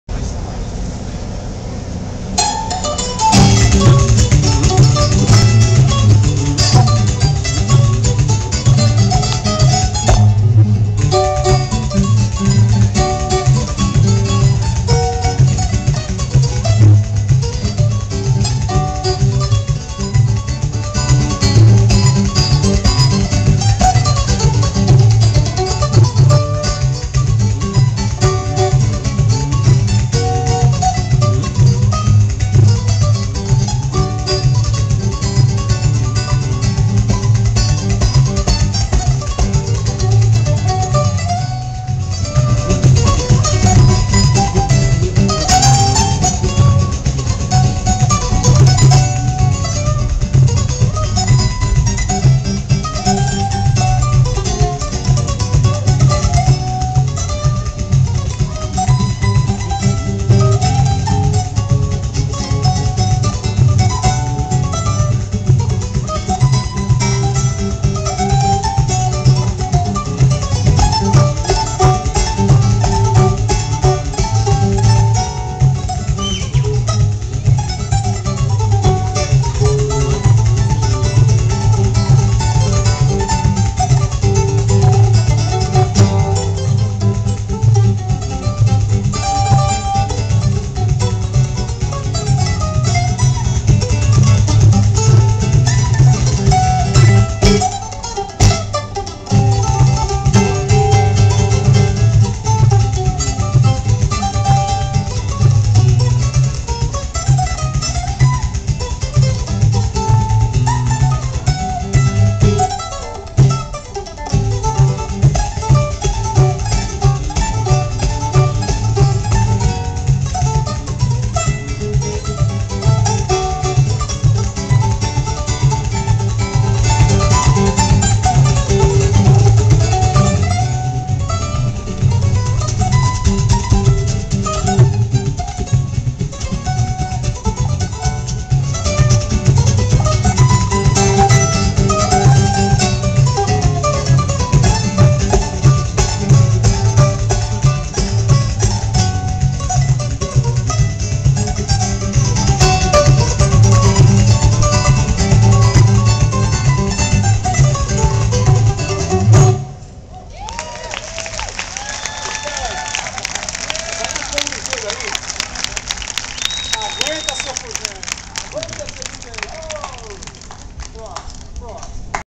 Vancouver International Jazz Festival - Jun 29 2008
Estanislau Gubiotti on Mandolin, Celso Machado on Guitar and Liam MacDonald on Percussion